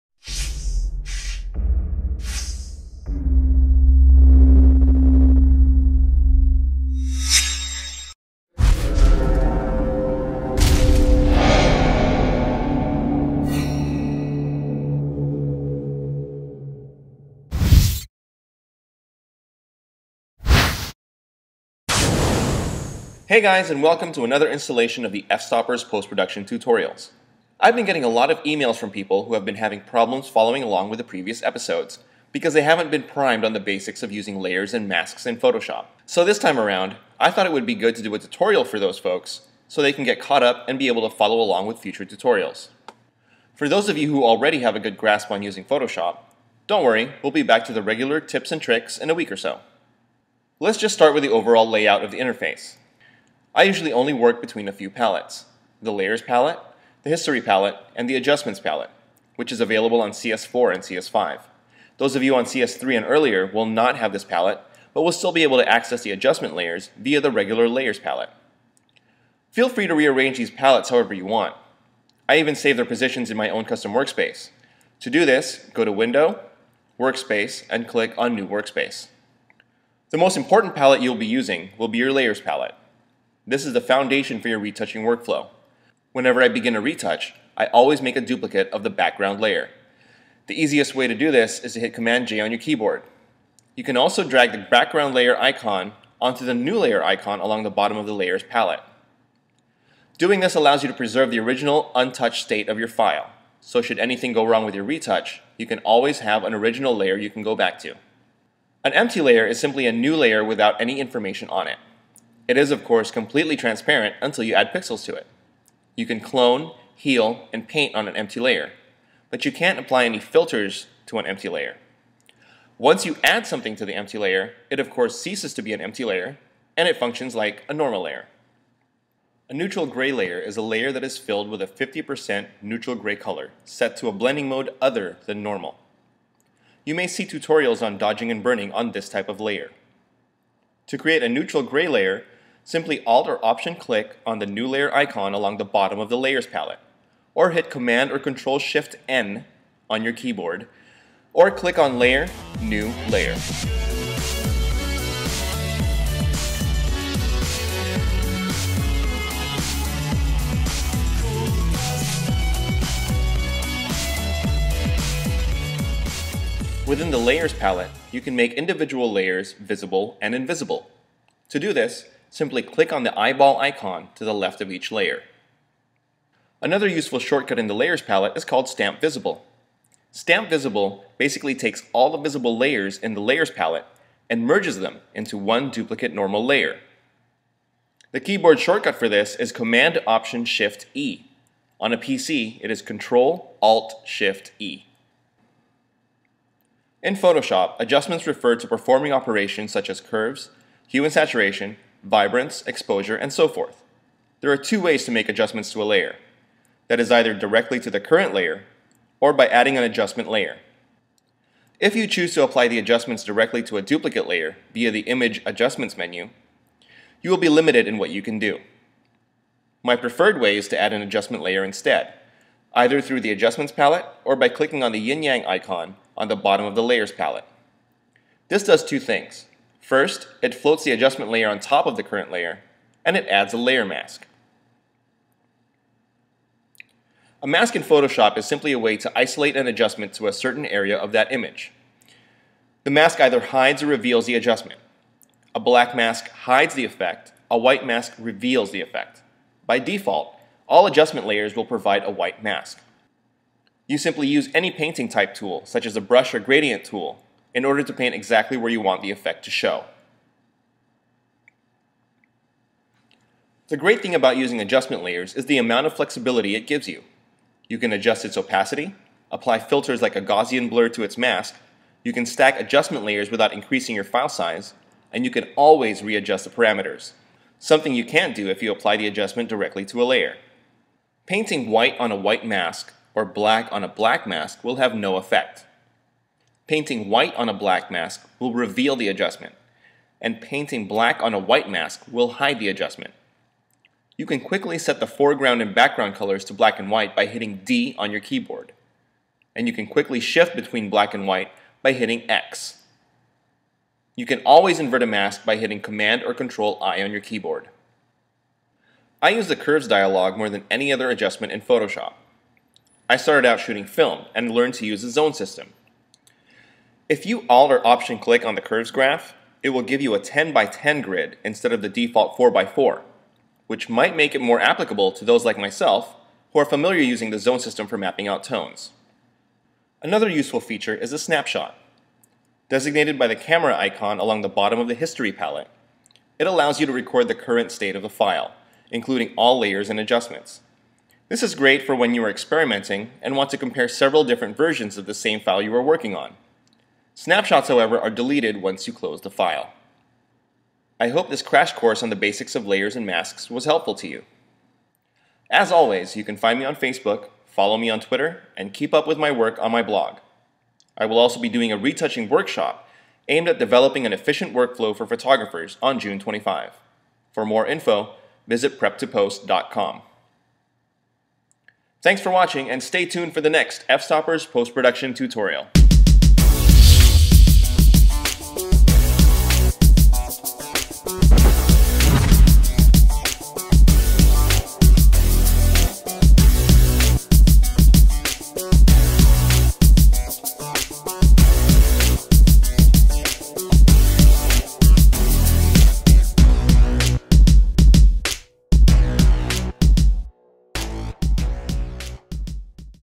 Photoshop 101: Basics of Layers and Masks by Sean Armenta
This is a crash course on the bare essentials of working with layers and masks in photoshop